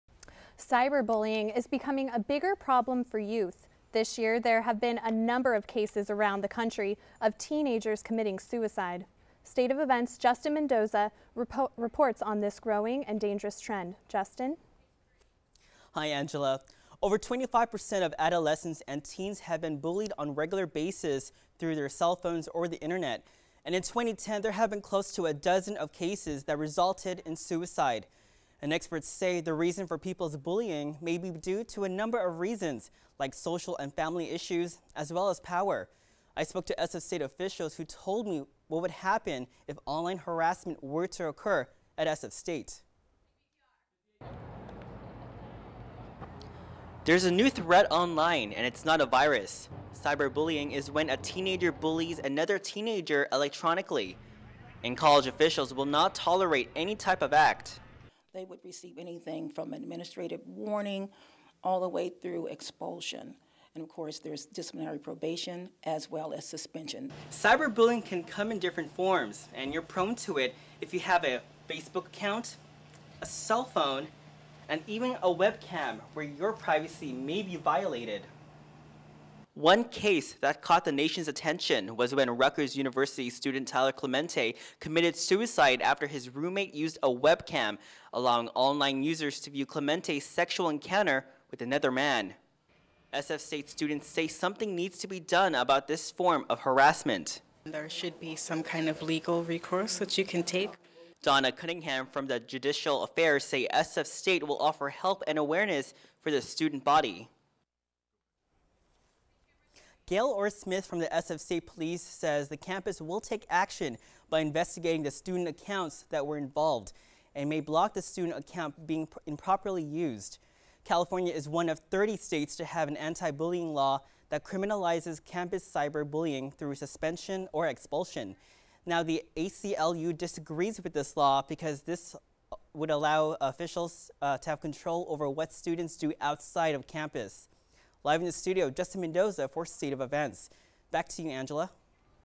Growing Cyber Bullying Trends
Justin Mendoza reports on the rise of cyber-bullying.